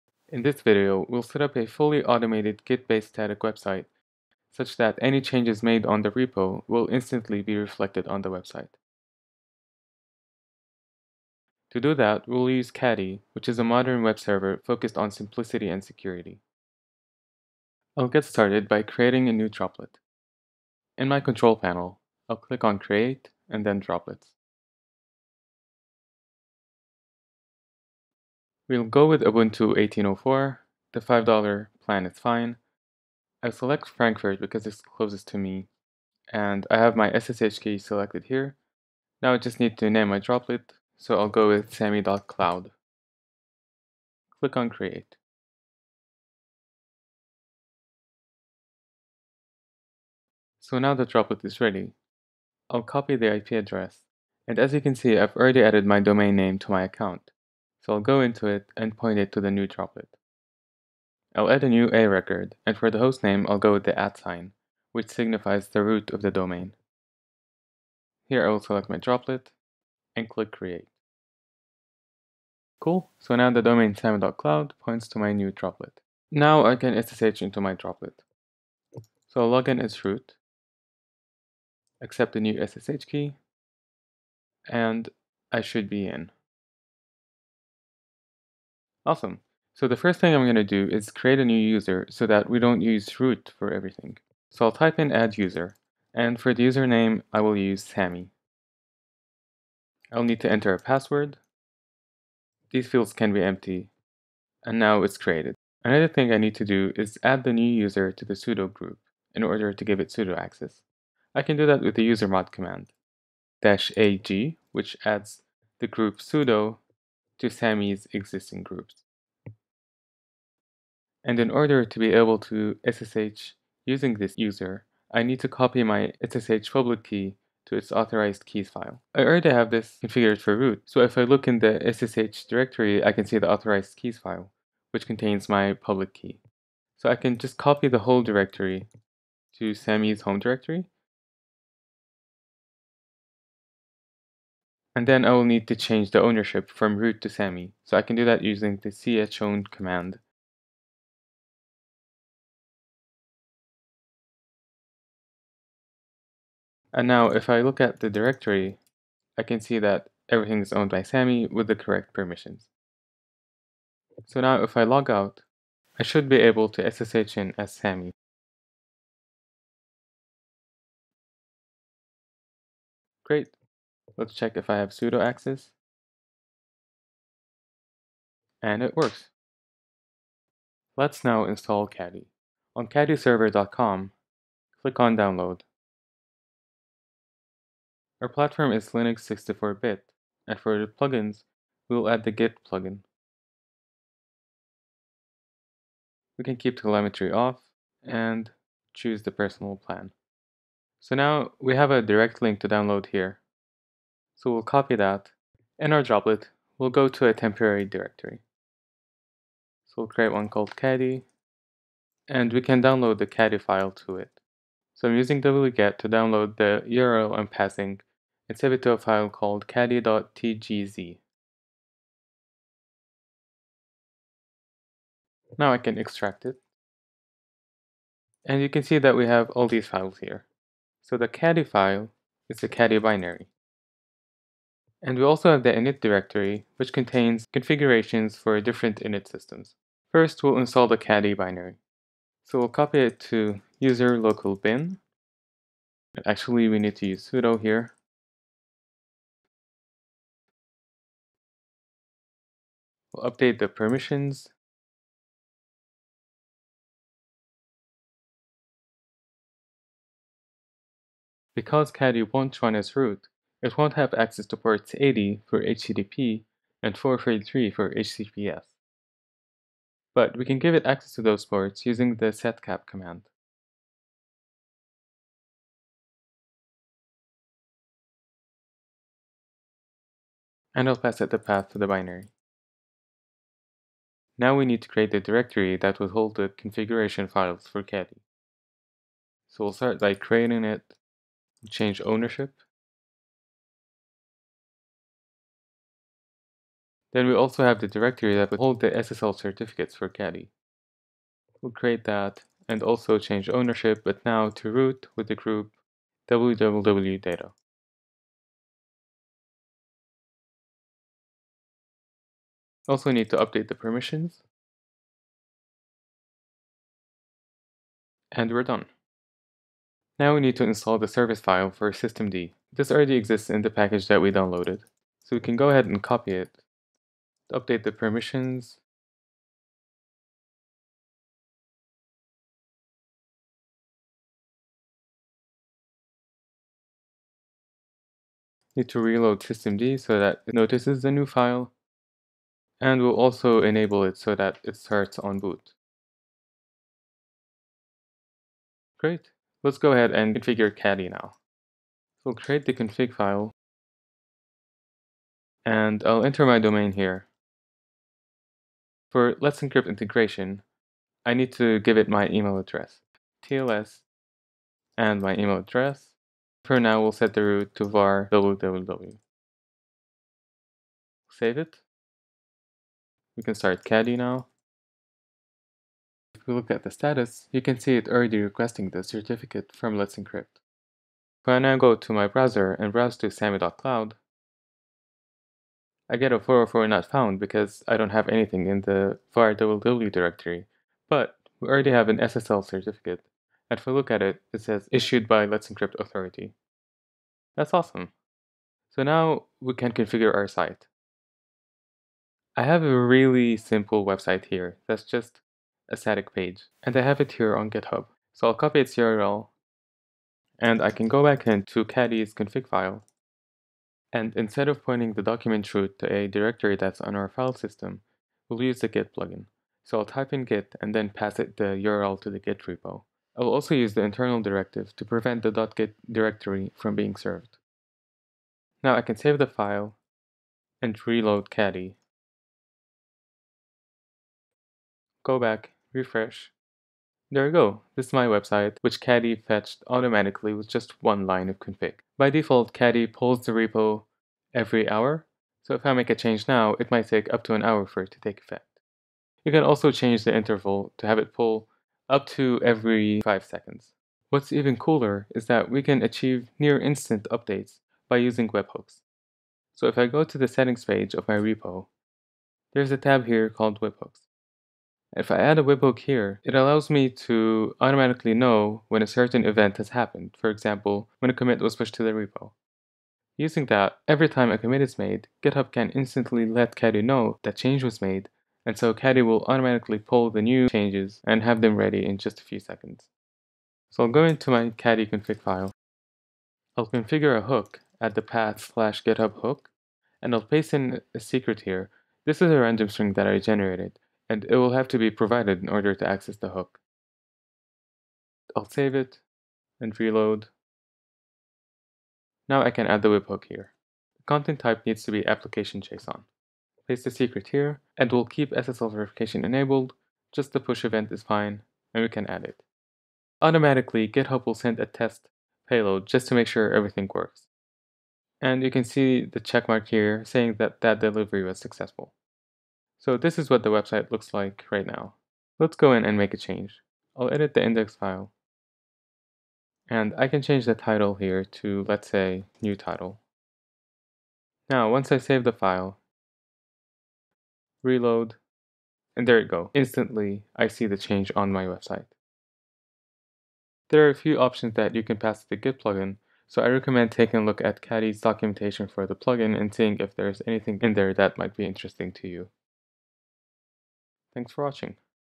How To Deploy a Fully Automated Git-synced Static Website Using Caddy
🔐 Generate a random secret using the `pwgen` command line tool